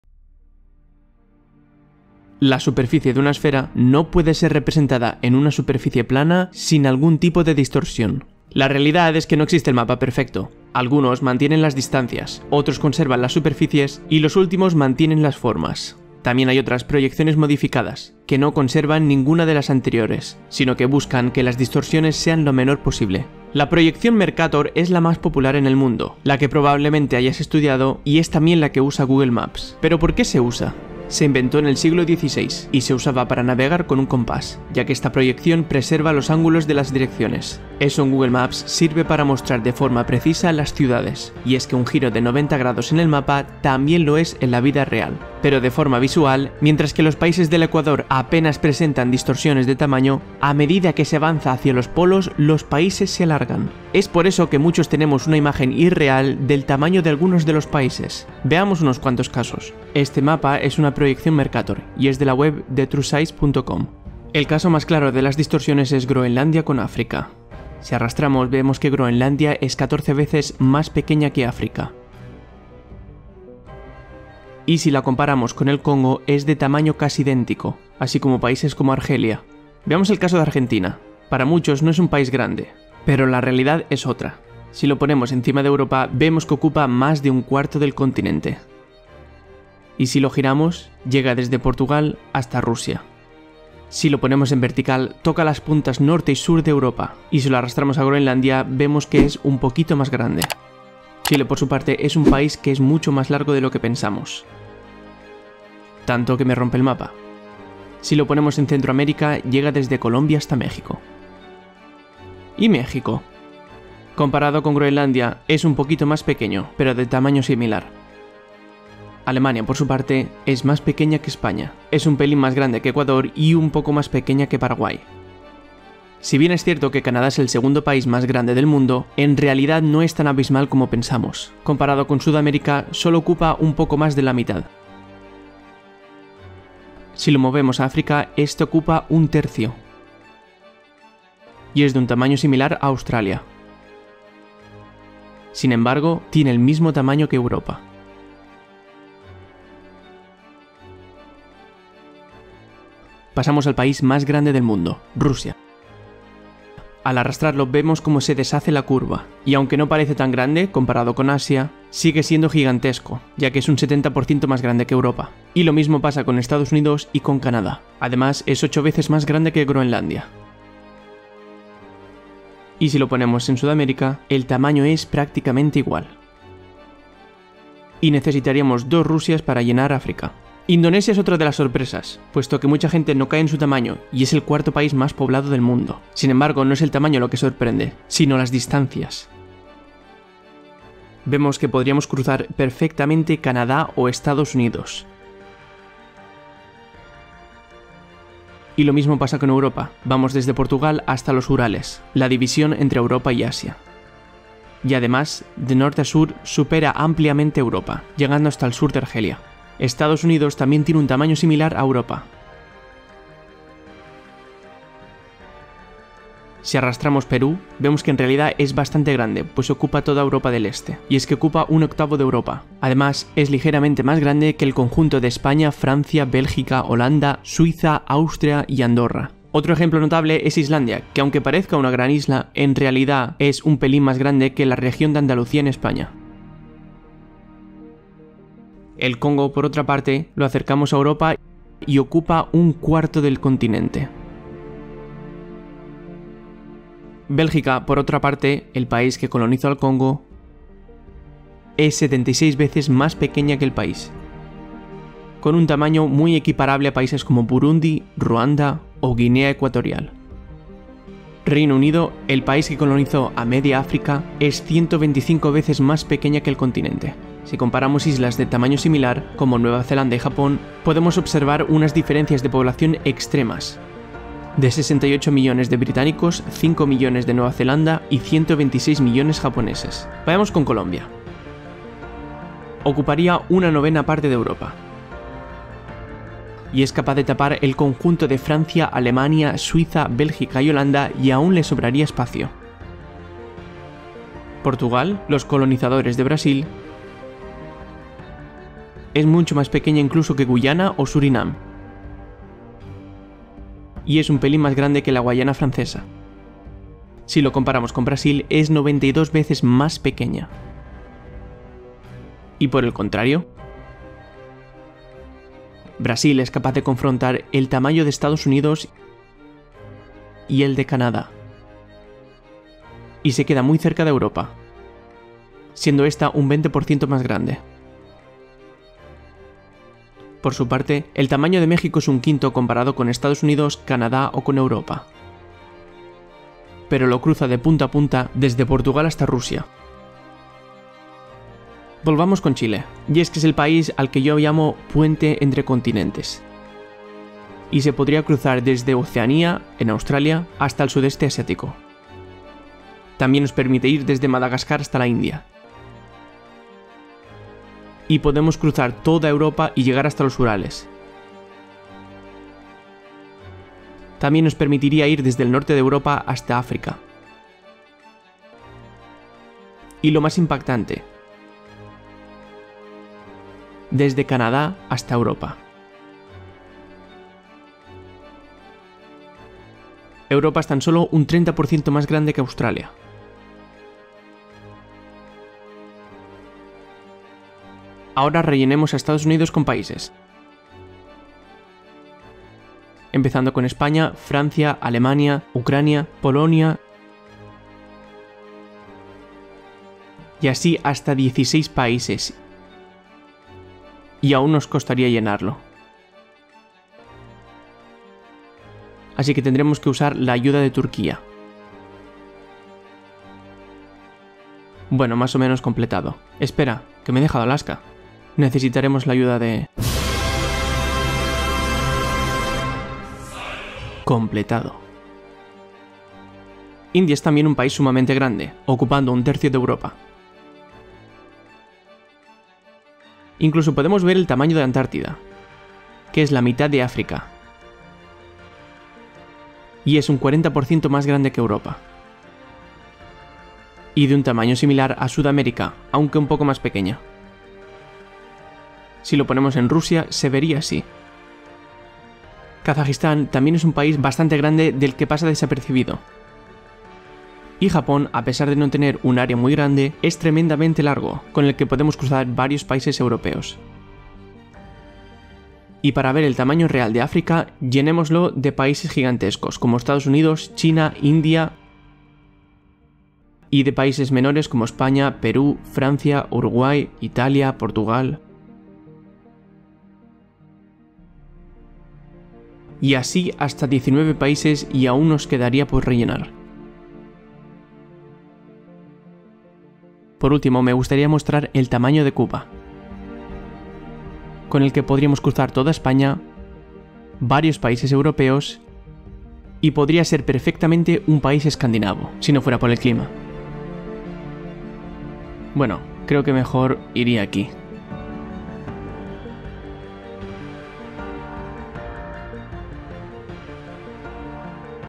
El TAMAÑO REAL de los PAÍSES
❓ ¿Qué pasaría si los países tuvieran un tamaño distinto al que pensamos? ✅

Pasar un mapa en 3D (el globo terráqueo) a un planisferio en 2D (un mapa) produce unas distorsiones que provocan que no exista el mapa perfecto. Algunos preservan las distancias, otros las formas (ángulos) y otros las superficies.

La proyección más usada en todo el mundo es la de Mercator, que es la que preserva las formas, y es también usada en Google Maps. Sin embargo, esto nos induce a errores a la hora de pensar en el tamaño de los países.

Hay países que no son tan grandes como pensamos (los que están al norte) y, por el contrario, hay países que nos sorprenden que sean tan grande (los que están alrededor del ecuador).

La comparativa más impactante es la de Groenlandia con África. En el mapa, ambos parecen de un tamaño muy similar. Sin embargo, la realidad es que África es 14 veces más grande. Esta y otras curiosidades, en el vídeo.